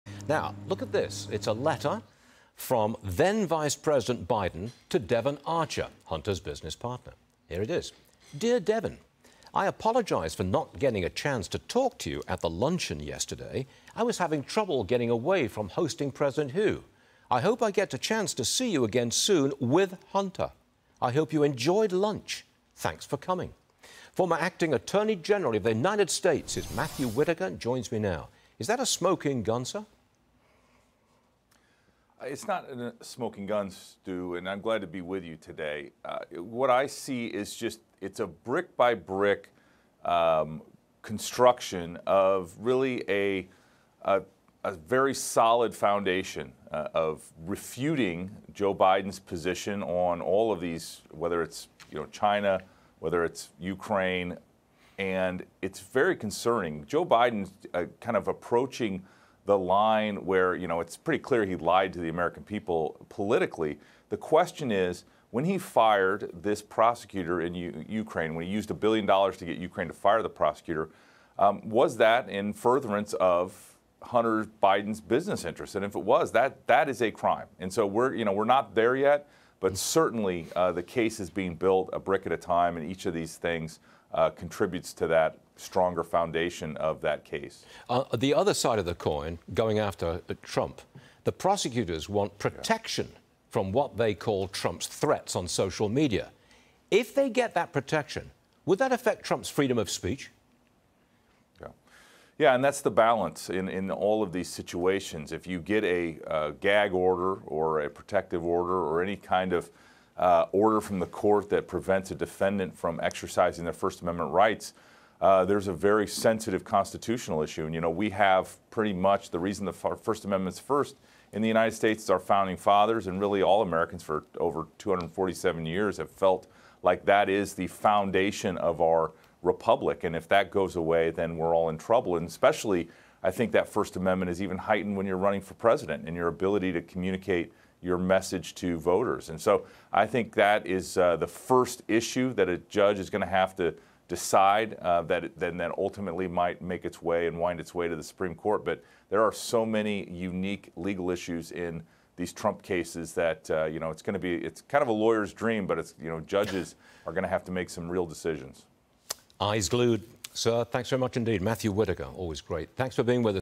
'DEAR DEVON': Is Biden's bombshell letter to Hunter's ex-biz partner a 'smoking gun'?
Former Acting Attorney General Matthew Whitaker discusses President Biden's connection to his son's business dealings, and the fallout from Trump's multiple indictments.